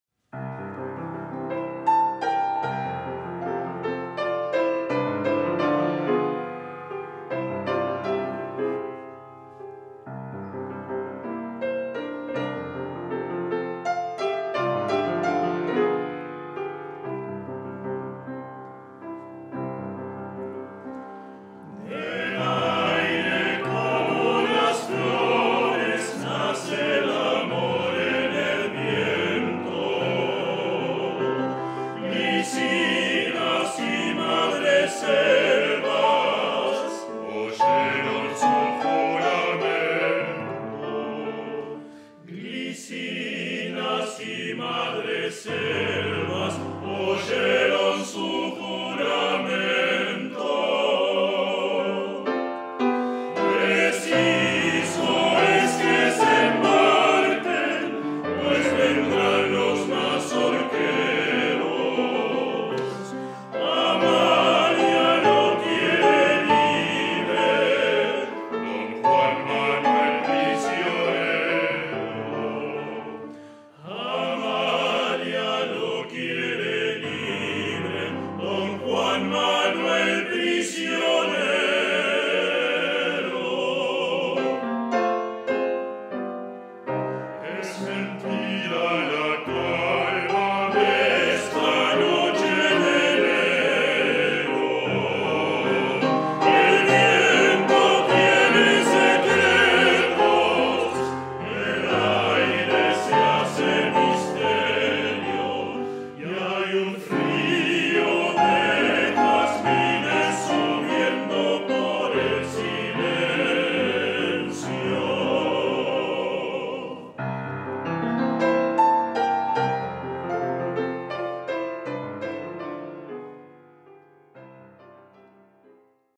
Coro Nacional | Latinoamerica coral
➡️ El Coro Nacional los espera el 12 de abril en el Auditorio Nelly Goitiño para disfrutar de "Latinoamérica coral", un homenaje a la rica tradición coral de nuestra región a través de compositores uruguayos, argentinos, brasileños, entre otros.